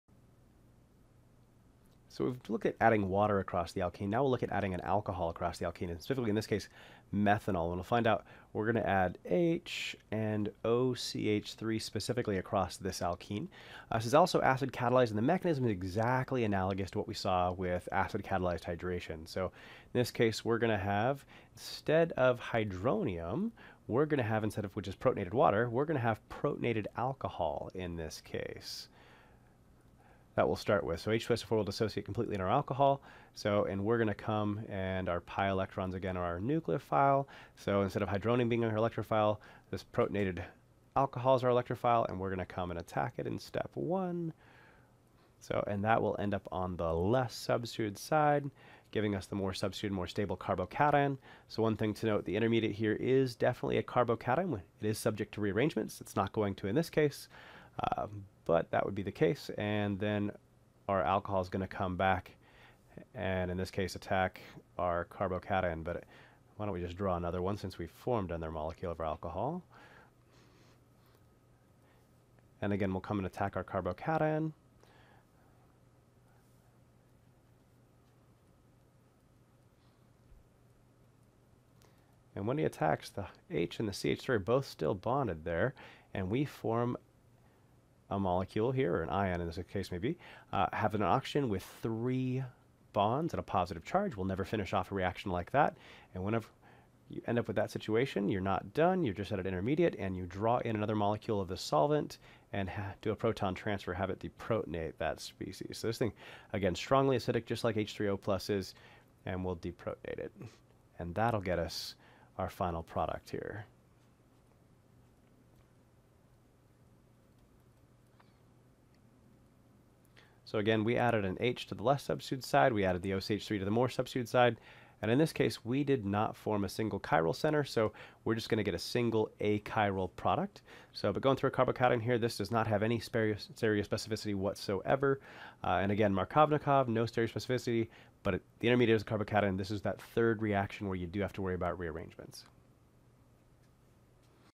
8.4a Acid Catalyzed Addition of an Alcohol
Chad breaks down Acid-Catalyzed Addition of an Alcohol (including the mechanism) including why it is Markovnikov addition with no Stereospecificity.

Happy Studying!